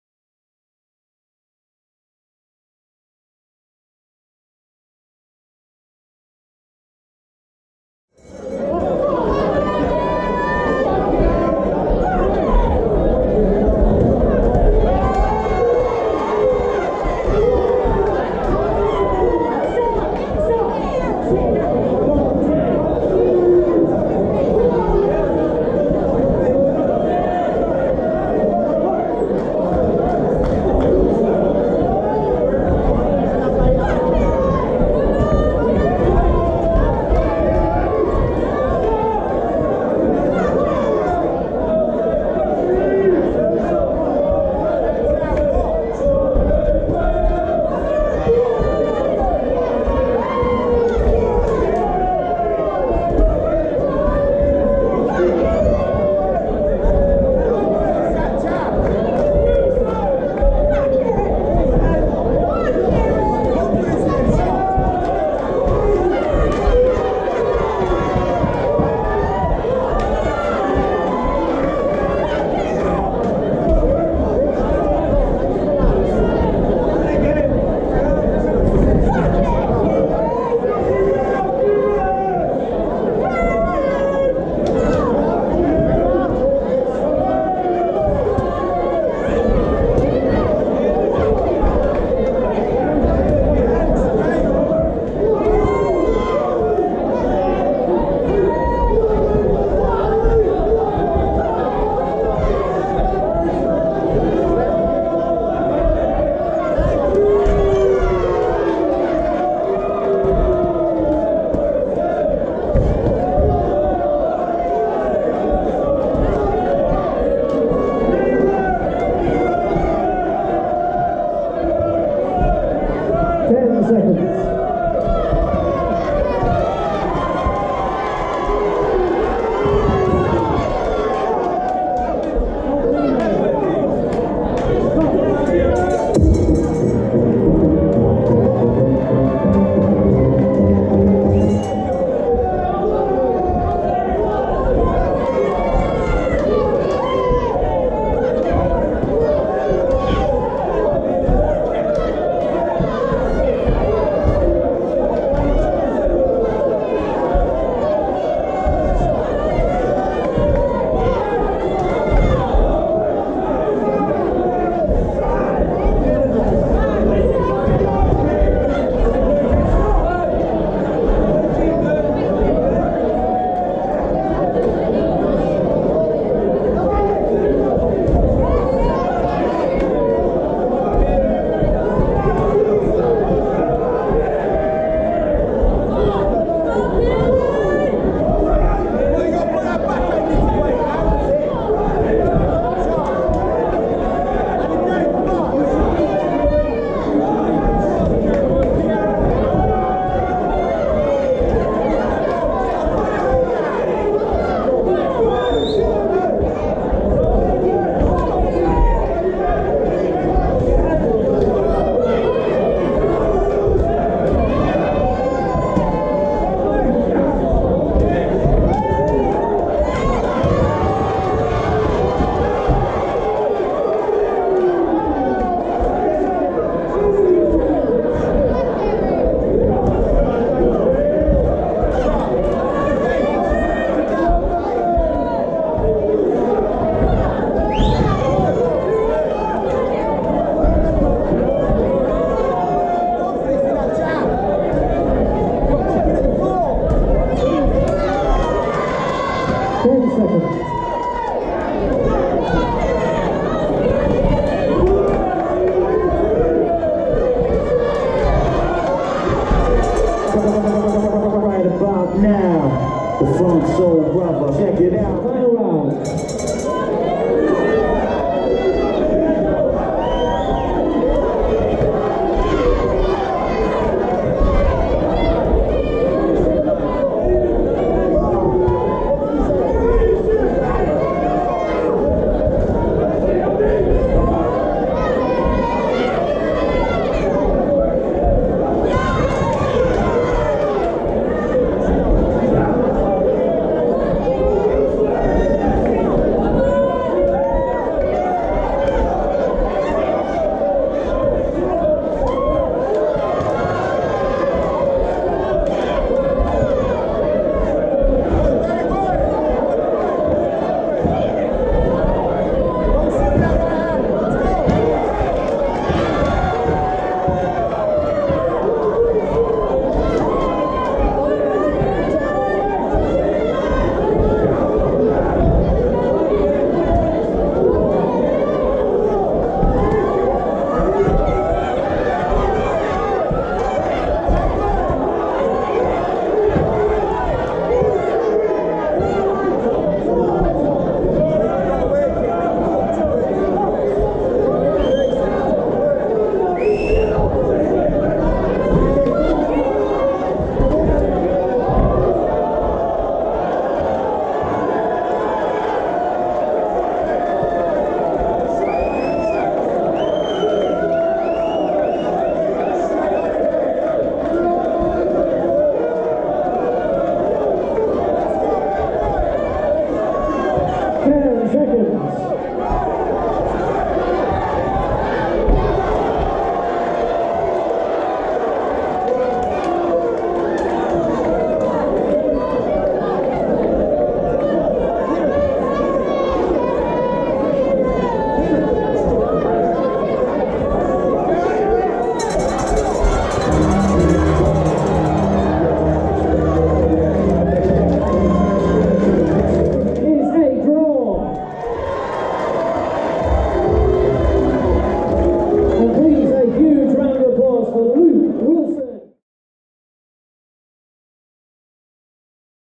Blackpool | Ultra White Coller Boxing | Luke wilson VS Kieron Skivington
Blackpool Ultra White Collar Boxing 1st September 2018

8 Weeks FREE training / Get in the best shape of your life/ Raise money for Cancer Research UK
/Plymouth at male & female beginners / Box at a glamorous event